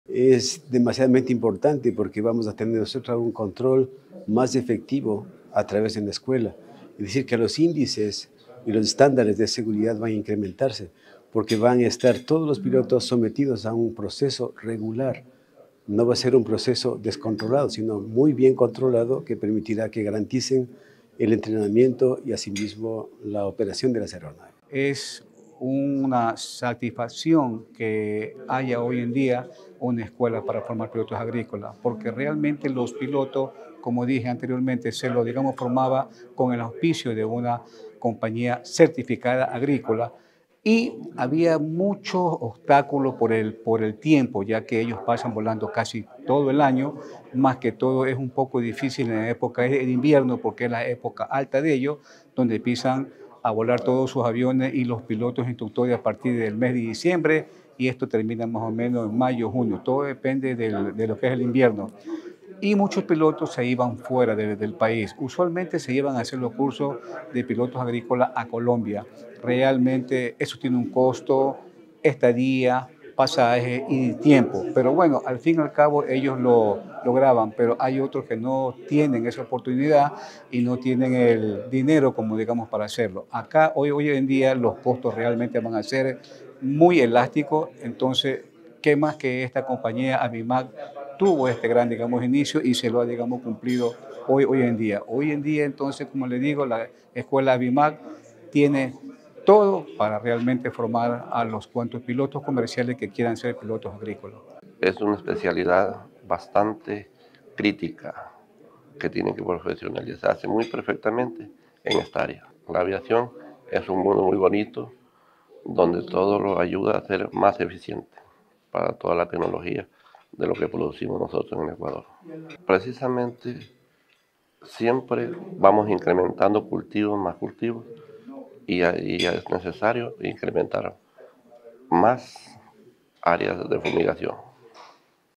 ECUADOR YA CUENTA CON SU PRIMERA ESCUELA DE PILOTOS PARA FUMIGACIÓN
“Es muy importante porque vamos a tener un control más efectivo a través de la escuela, los estándares de seguridad van a incrementarse porque todos los pilotos estarán sometidos a un proceso regular, muy bien controlado que permitirá el entrenamiento y así mismo la operación de las aeronaves” señaló.